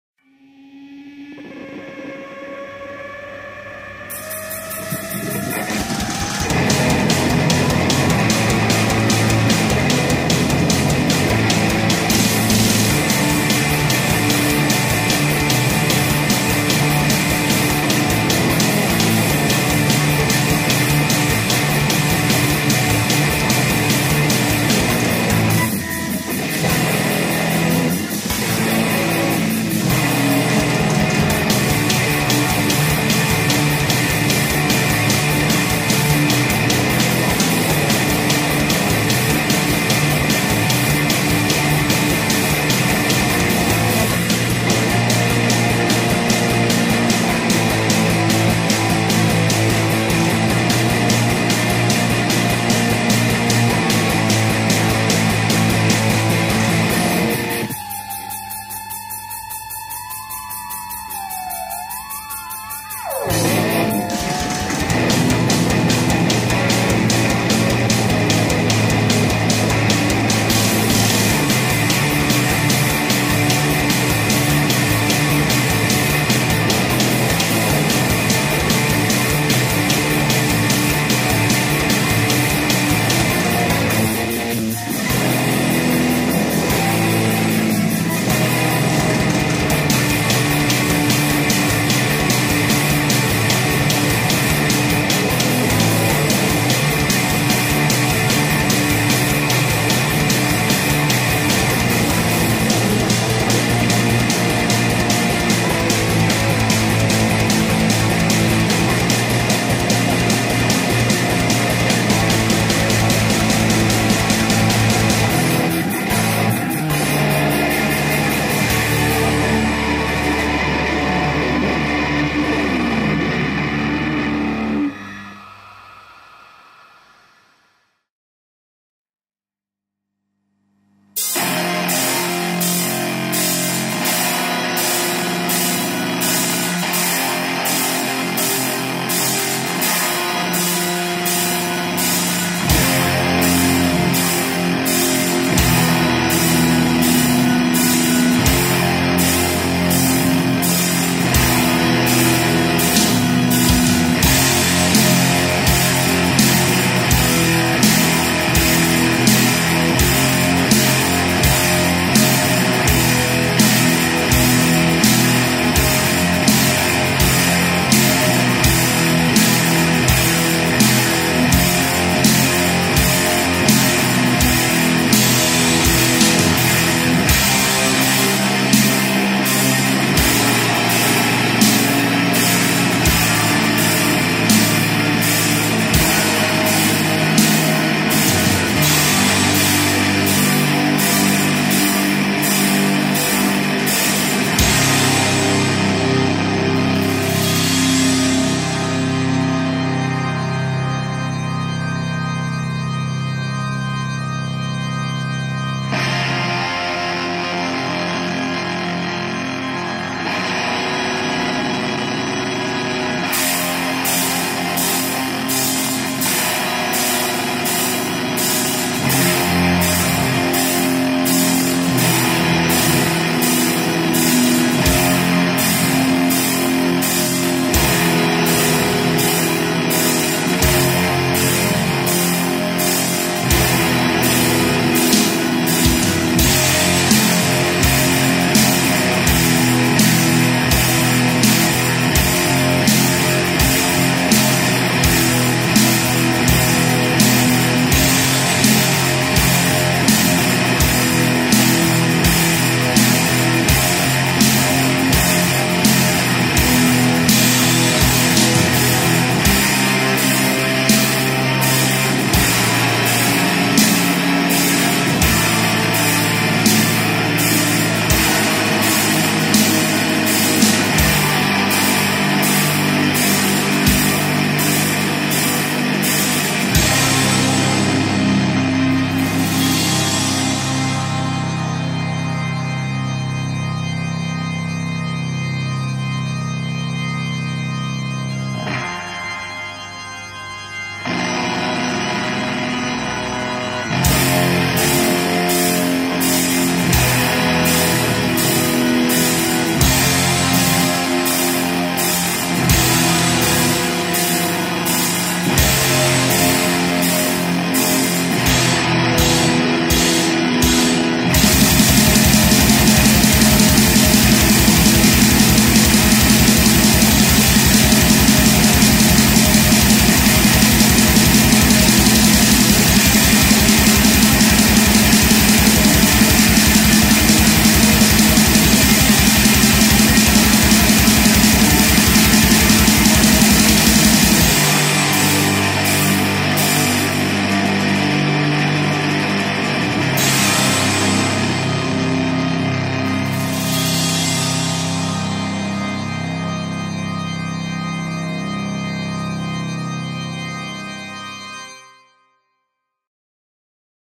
The Ayn Rand band present 2 unfinished songs.
0.00 Seaside postcard thrash'n'roll
2.04 Blackened doom
Why not join in the fun? Write some lyrics and send me a .wav recording of  the vocal performance of one or both of these tracks. Cheers.